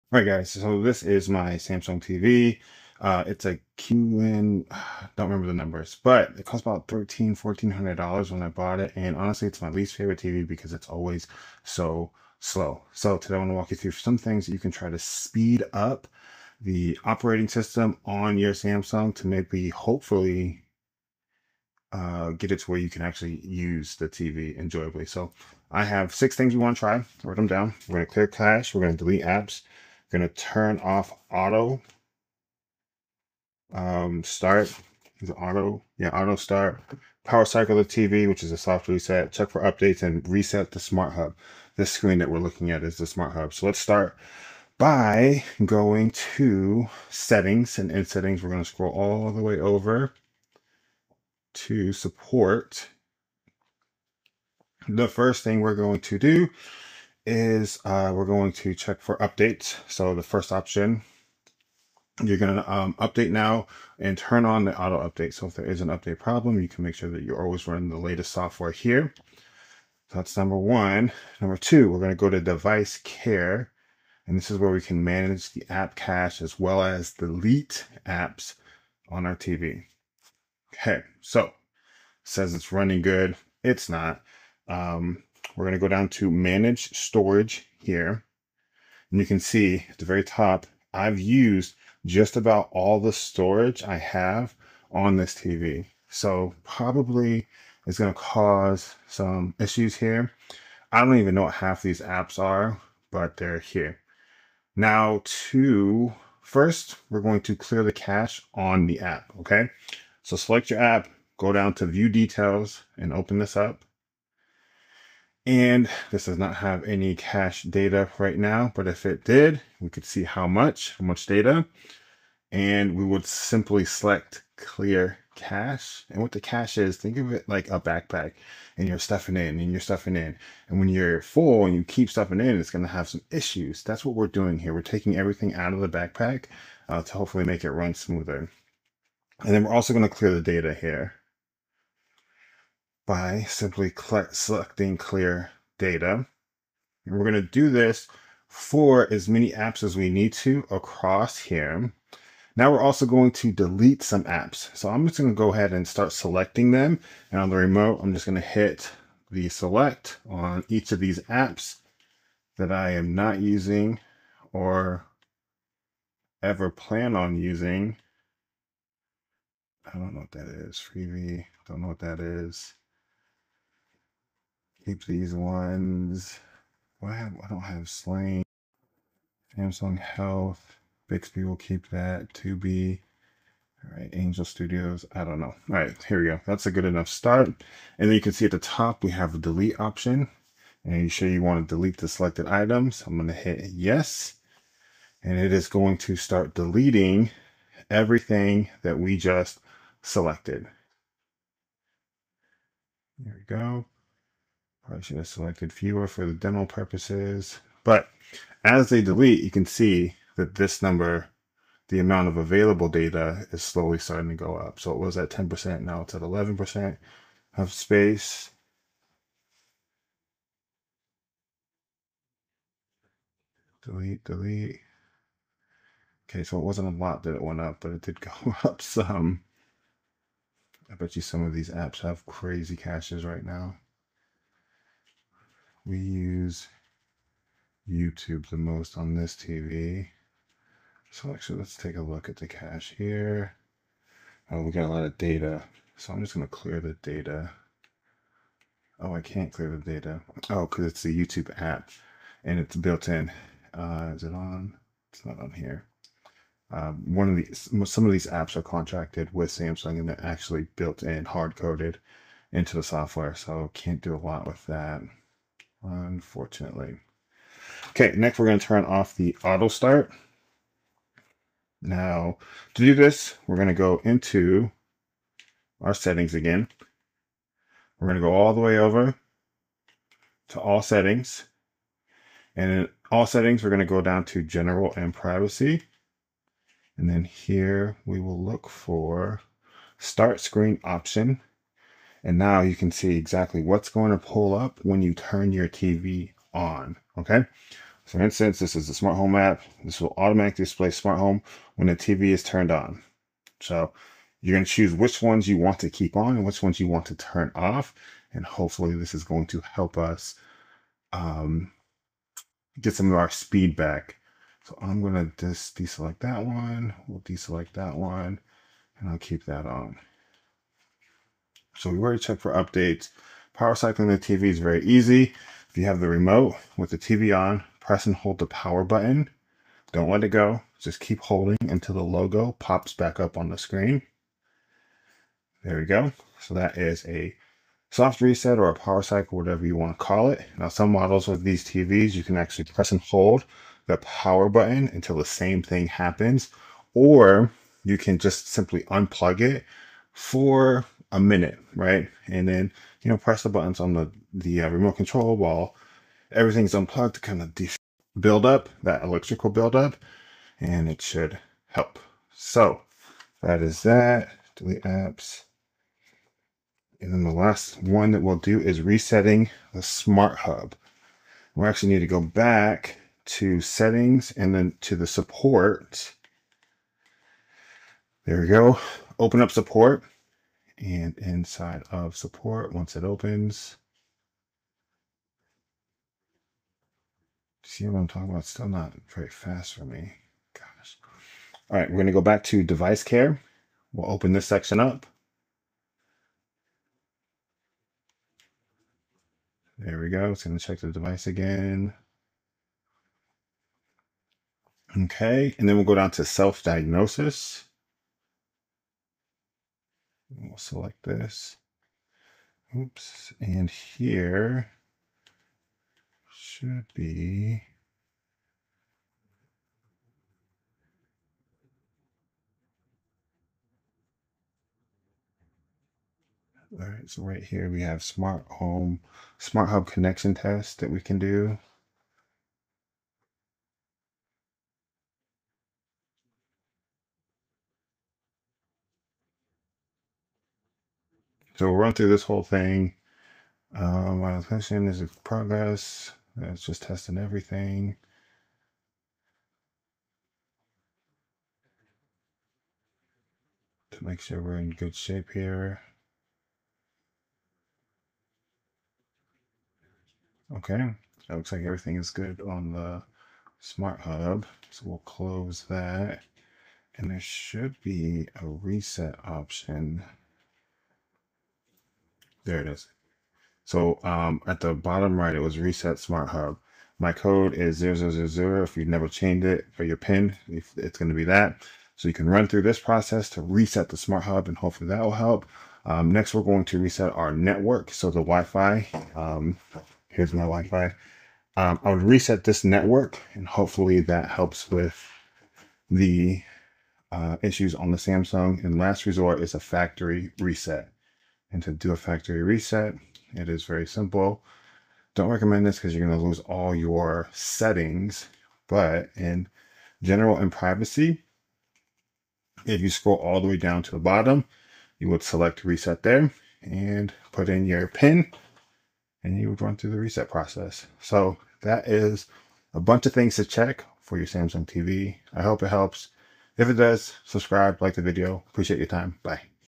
How To Speed Up A Slow Samsung TV
Is your Samsung Smart TV running slow or lagging? This video shows you simple steps to speed up your Samsung TV for smoother performance. Works on: Samsung Smart TVs.